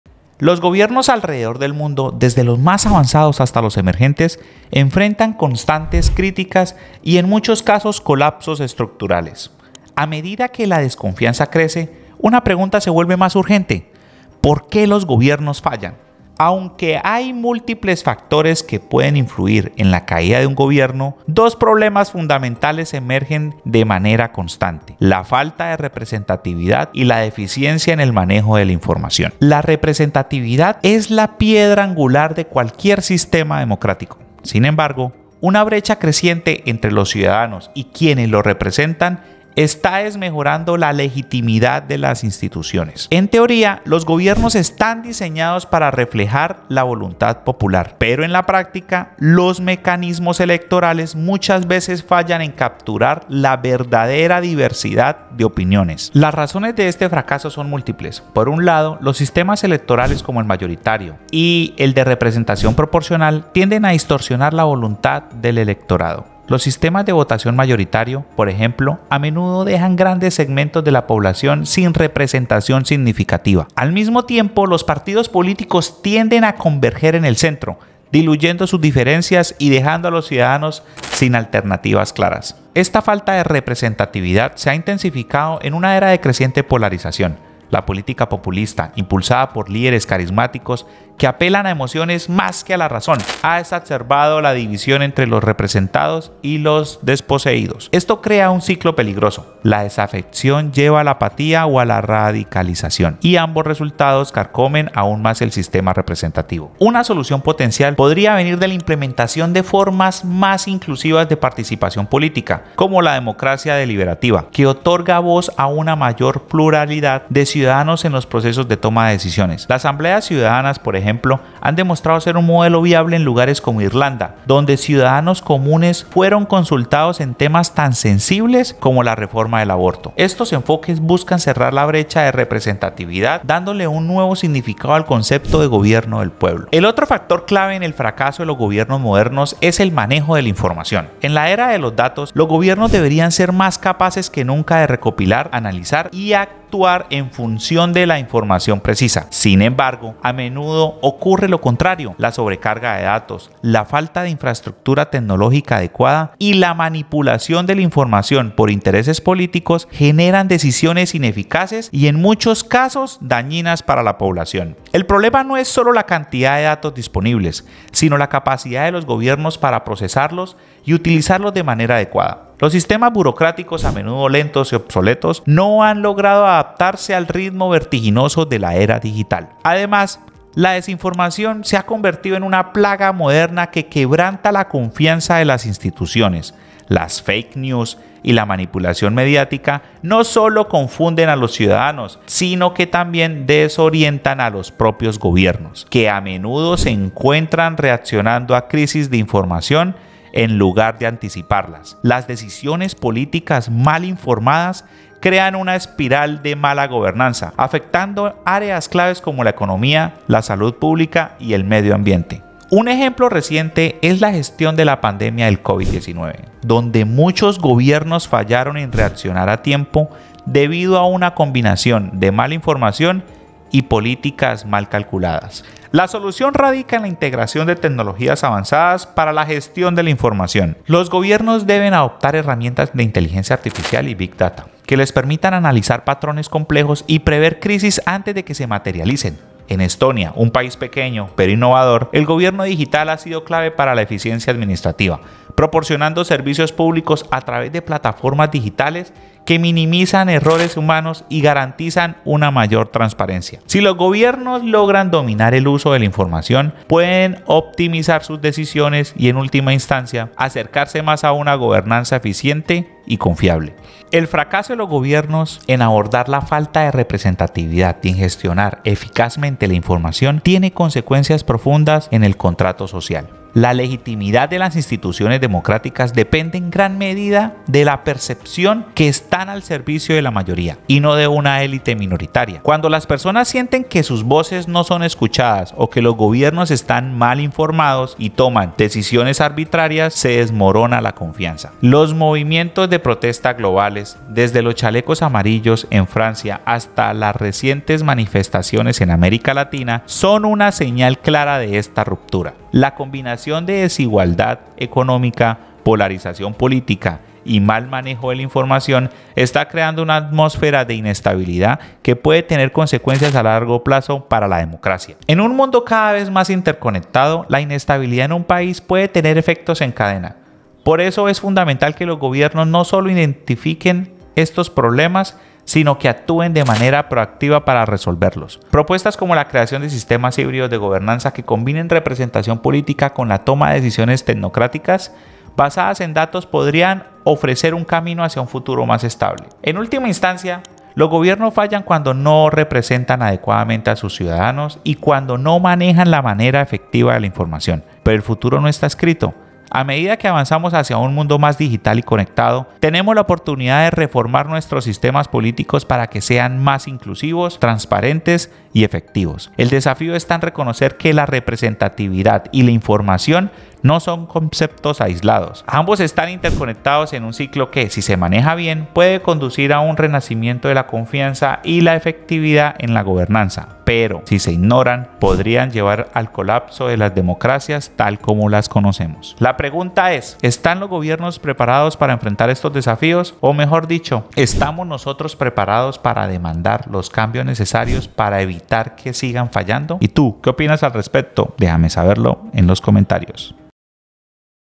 #155 ¿Por qué los Gobiernos Fallan? Representatividad y Mala Gestión de la Información
¿Por qué los gobiernos alrededor del mundo están fallando? En este video analizamos dos problemas fundamentales: la falta de representatividad y la mala gestión de la información. Exploramos cómo los sistemas democráticos distorsionan la voluntad popular, cómo la polarización política está afectando la participación ciudadana y por qué la desinformación está debilitando las decisiones gubernamentales. Además, mostramos soluciones como la democracia deliberativa y el uso de tecnologías avanzadas en la administración pública. jesusbaront podcast conectadoscon Gobernanza, política, democracia, representatividad, desinformación, inteligencia artificial, polarización, elecciones, fake news, tecnología, participación ciudadana, big data, democracia deliberativa.